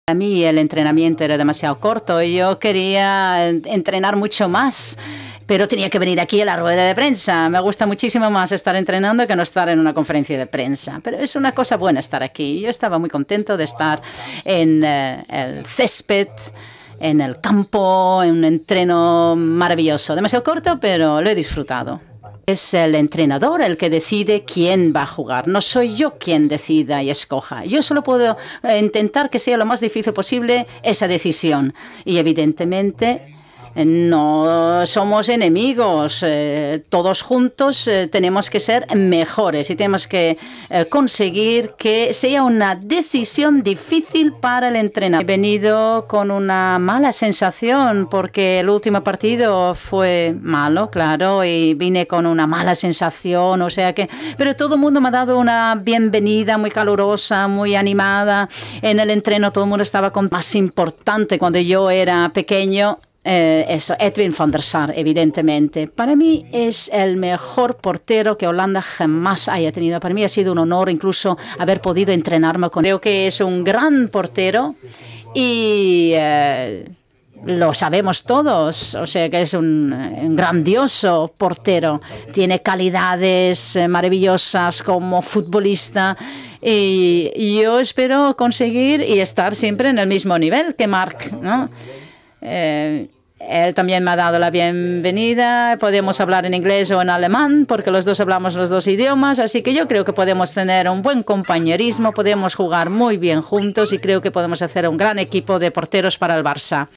Cillessen: "Ter Stegen y yo no somos enemigos"
Jasper Cillessen ya ha sido presentado como jugador azulgrana y ha asegurado que entre él y Marc-André Ter Stegen habrá una competencia sana.